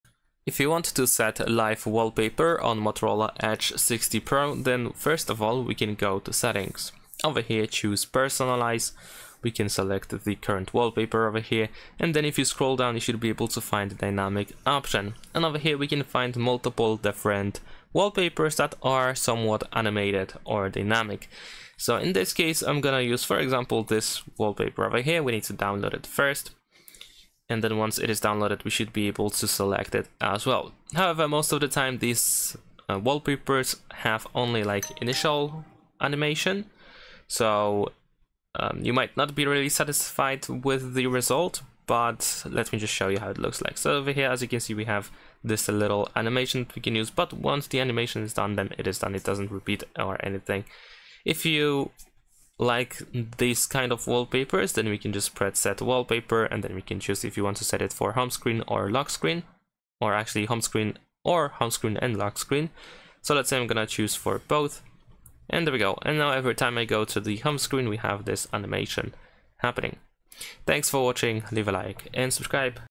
Motorola Edge 60 Pro: How to Set Live Wallpaper (Animated Wallpaper)
Make your Motorola Edge 60 Pro stand out with dynamic live wallpapers! This tutorial shows you how to set animated wallpapers step by step for a more vibrant home screen. Learn where to find built-in live wallpapers and how to apply third-party animated backgrounds easily. Transform your device's look with these eye-catching motion effects today!